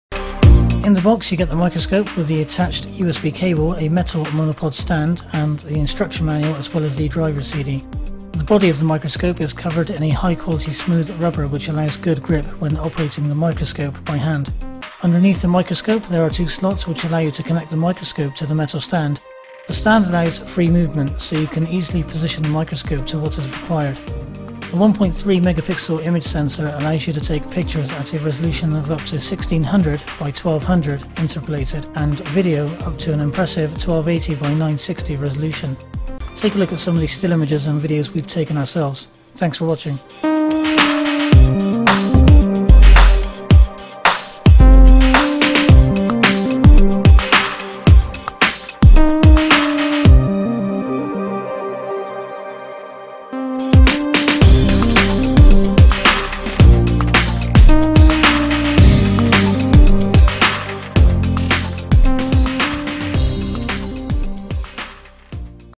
Veho Microscope
The latest USB Microscope from Veho. Now with more powerful 400x magnification, 2 Mega Pixel Cmos lens (interpolated) and alloy flexi-stand and measurement software...